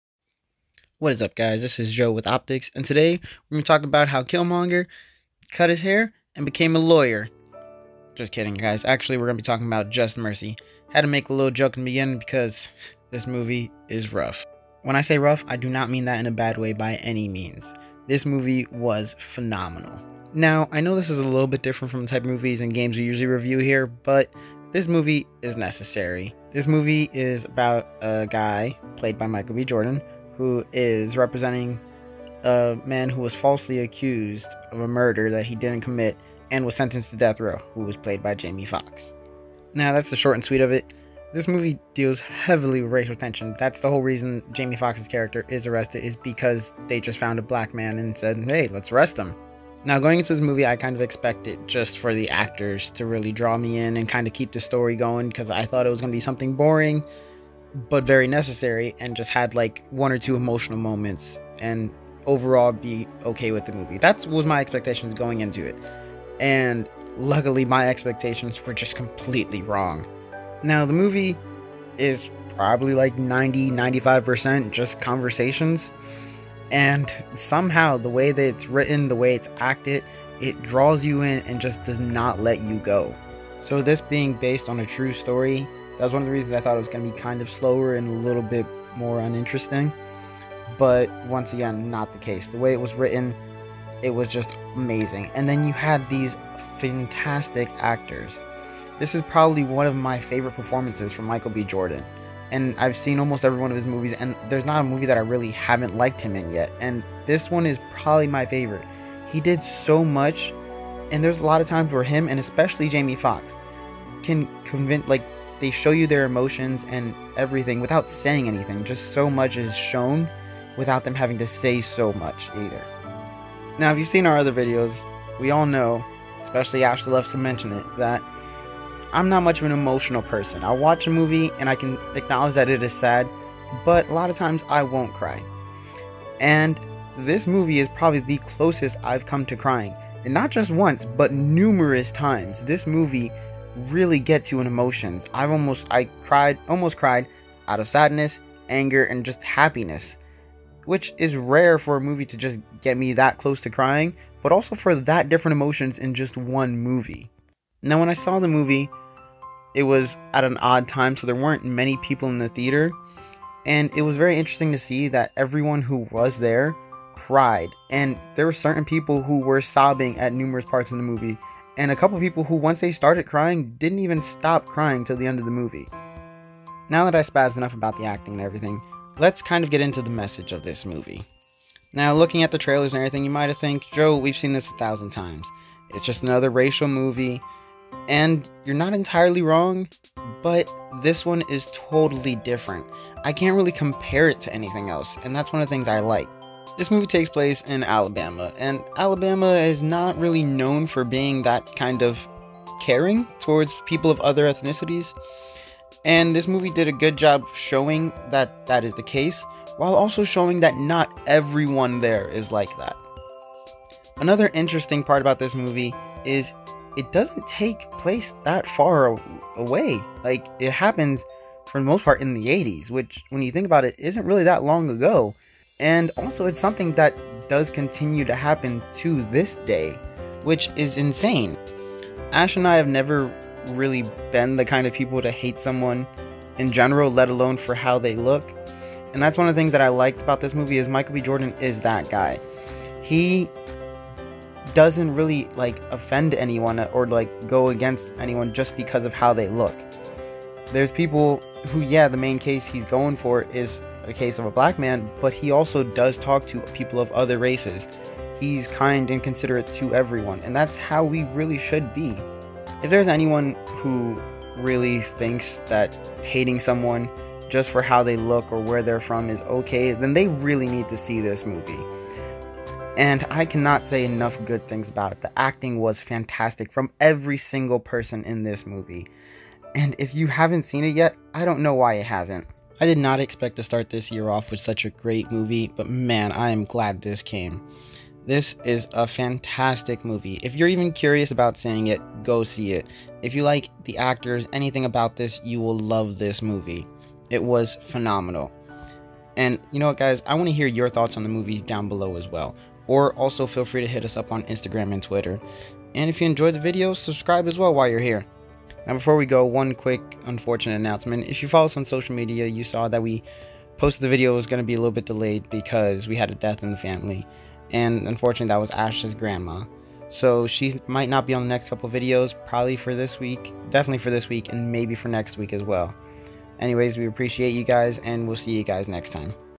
Just Mercy Review
Order in the court!! Today we review Just Mercy!